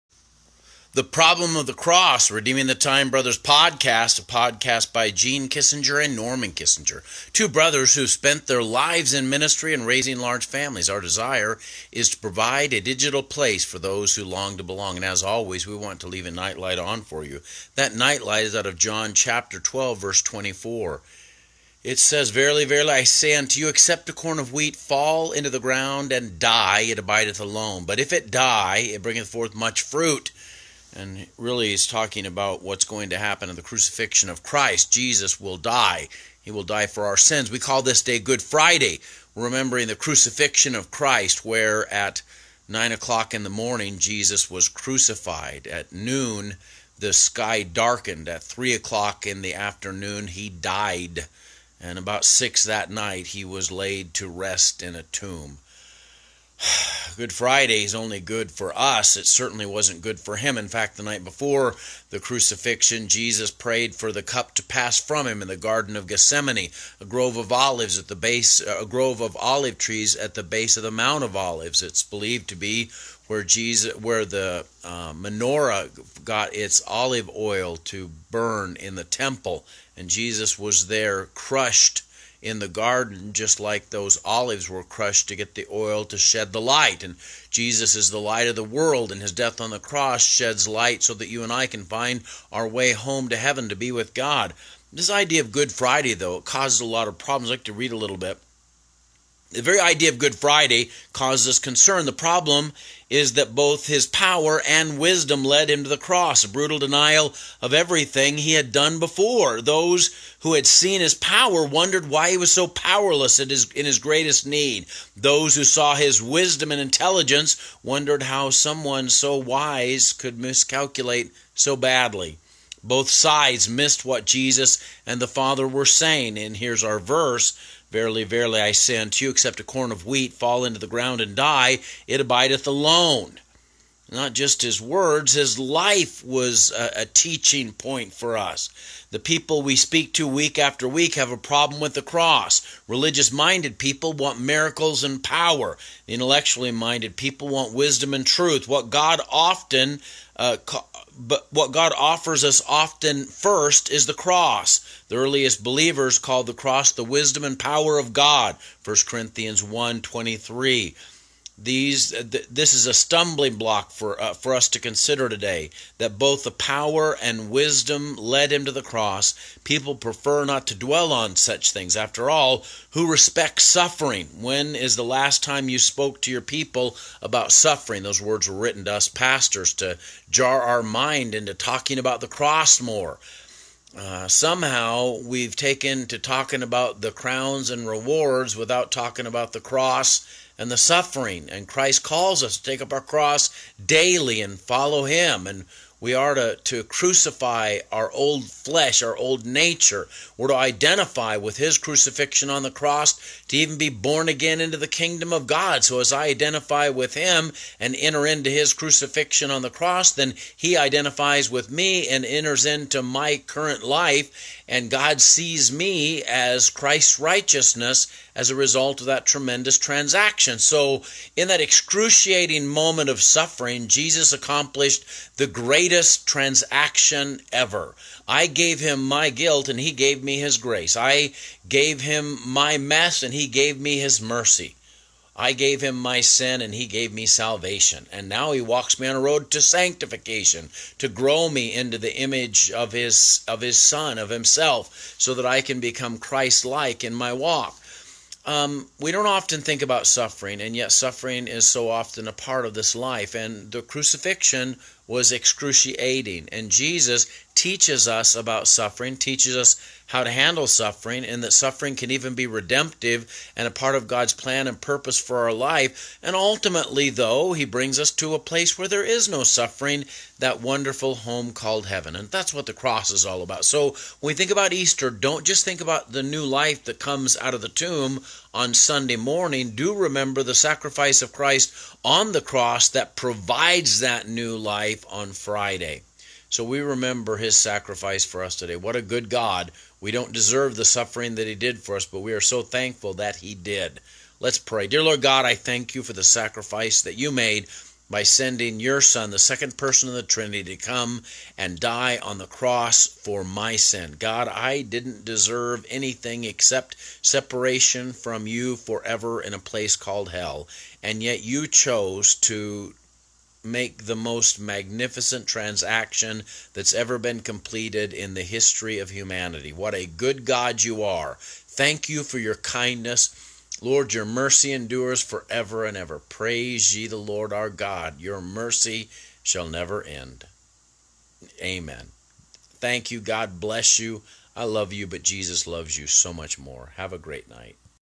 The Problem Of The Cross John 12:24
If Jesus was all-powerful why didn't he come down from the cross. If Jesus was all knowing how come he didn't find another way besides the cross. the truth of the matter is the cross was God's idea from eternity past.  The Bible describes Jesus as the lamb slain from the foundation of the world.  In the cross and all holy God became himself a sacrifice for his sin shattered creation.
On the Cross the Greatest transaction in human history took place, I gave him my sin and guilt and He gave me His Salvation and Grace.